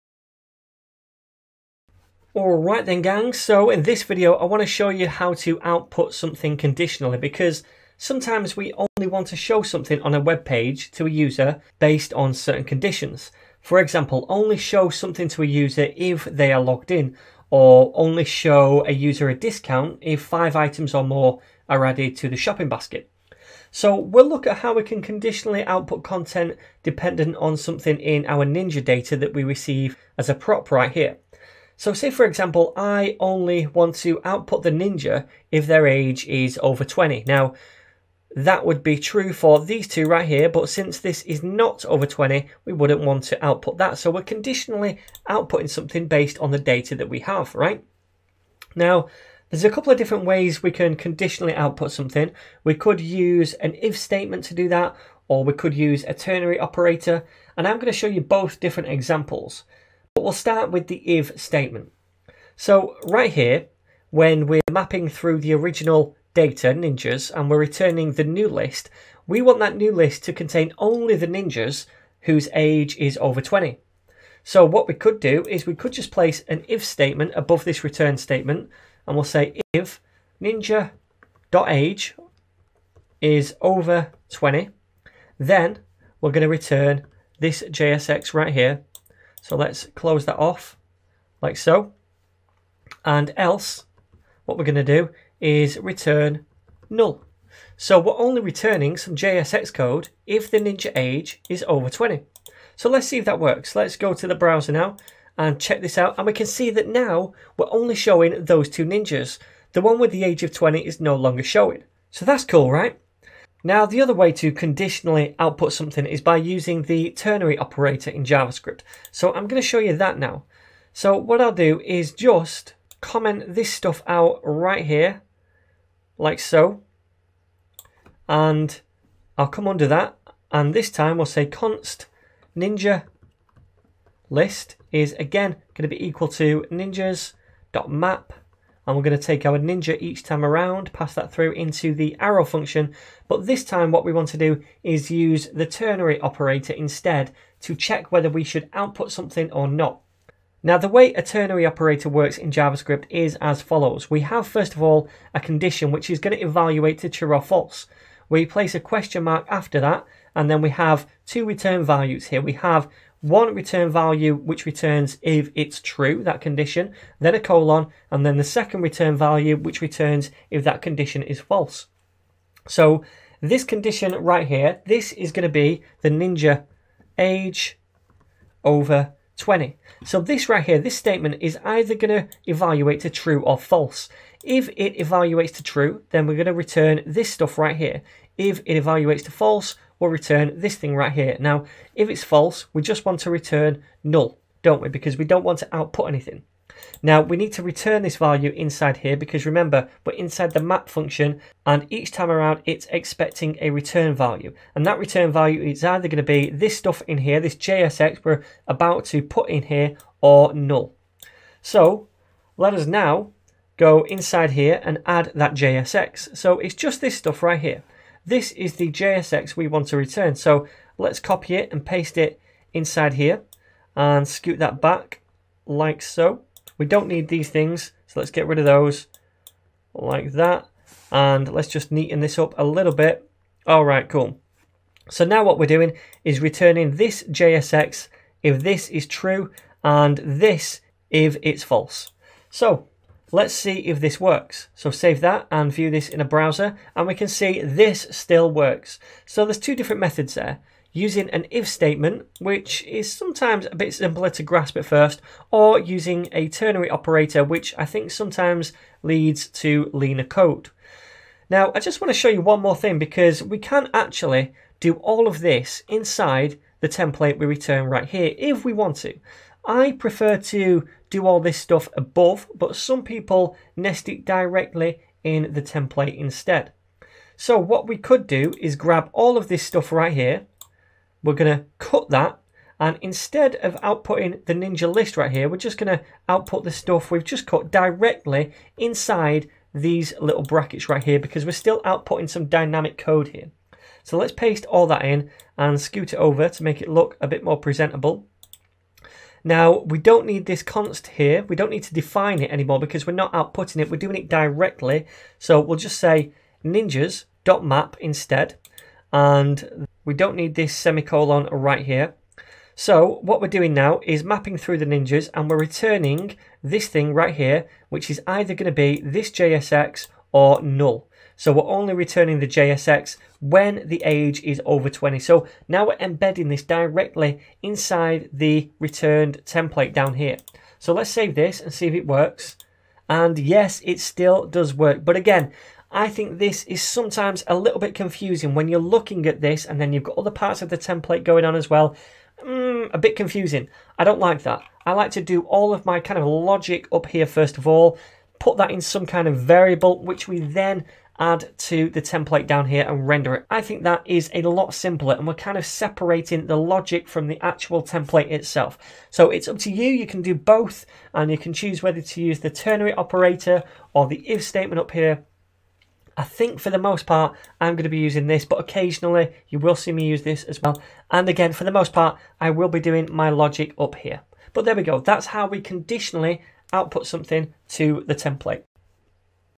Complete React Tutorial (& Redux) #16 - Conditional Output
Hey gang, in this React tutorial I'll be showing you how to conditionally output parts of our template dependent on certain conditions within our data.To do this we'll use a combo of if statements and ternary operators.